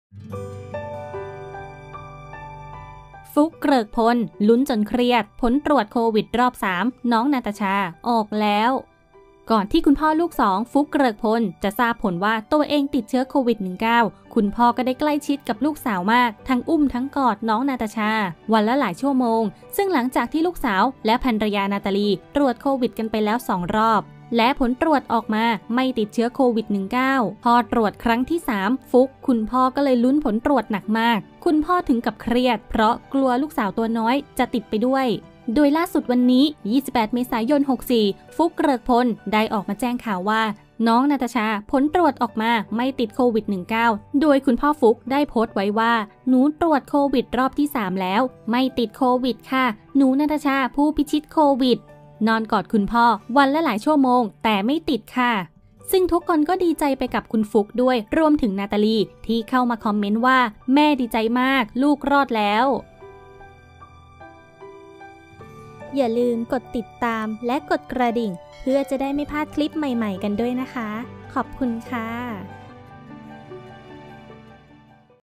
"ฟลุค เกริกพล" ลุ้นจนเครียด ผลตรวจโควิดรอบ 3 "น้องนาตาชา" ออกแล้ว
ก่อนคุณพ่อลูกสอง ฟลุค เกริกพล จะทราบผลว่าตัวเองติดเชื้อโควิด-19 คุณพ่อก็ใกล้ชิดกับลูกสาวมาก ทั้งอุ้มทั้งกอด น้องนาตาชา วันละหลายชั่วโมง ซึ่งหลังจากลูกสาว และภรรยา นาตาลี เจียรวนนท์ ตรวจโควิดกันไปแล้ว 2 รอบ และผลตรวจออกมาไม่ติดโควิด-19

พอตรวจครั้งที่3 ฟลุค คุณพ่อก็เลยลุ้นผลตรวจหนักมาก คุณพ่อถึงกับเครียดเพราะกลัวลูกสาวตัวน้อยจะติดไปด้วย แล้วล่าสุดวันนี้ (28 เม.ย. 2564) ฟลุค เกริกพล ก็ได้ออกมาแจ้งข่าวว่า น้องนาตาชา ผลตรวจออกมาไม่ติดโควิด-19 ตามที่ฟลุคได้โพสต์ไว้

"หนูตรวจ Covid รอบ 3 เเล้ว หนู babynatashacm เป็น supergirl ไม่ติด Covid คะ  หนูนาตาชาผู้พิชิตโควิด นอนกอดคุณพ่อวันละหลายชั่วโมงเเต่ไม่ติดคะ"

ซึ่งทุกคนก็ดีใจไปกับฟลุคด้วย รวมถึง  นาตาลี ที่เข้ามาคอมเมนต์ว่า "แม่ดีใจมากกกกลูกรอดแล้ว"